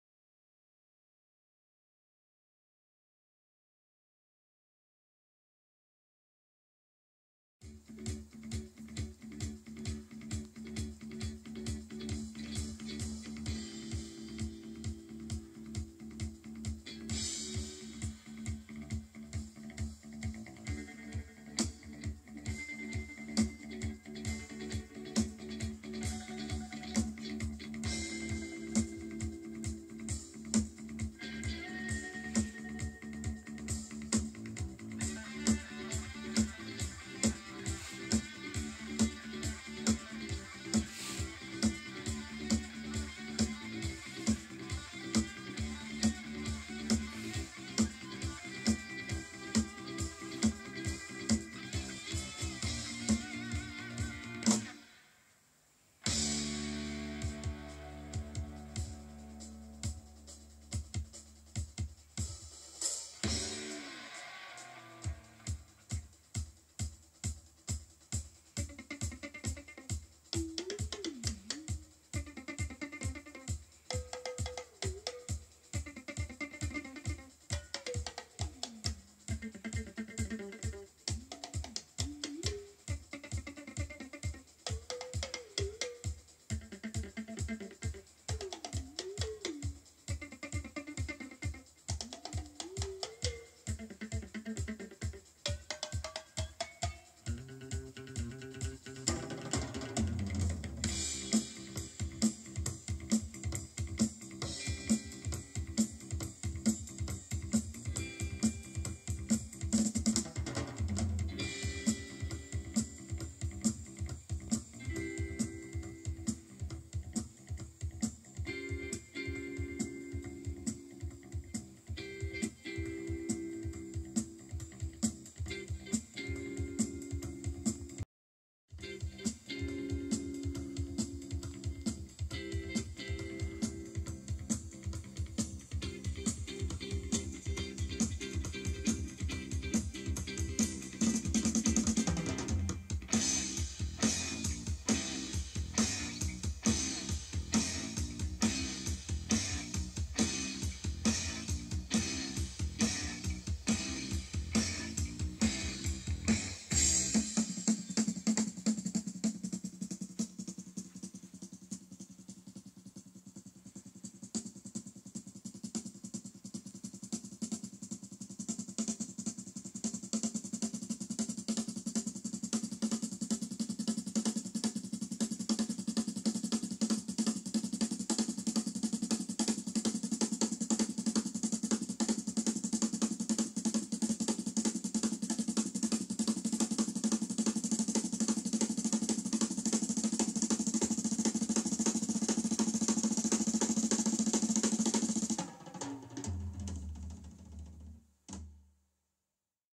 Braun C3 - Vintage Tapedeck Sound Example
A sound example recorded on Scotch Metal Tape Cassette without noise reduction.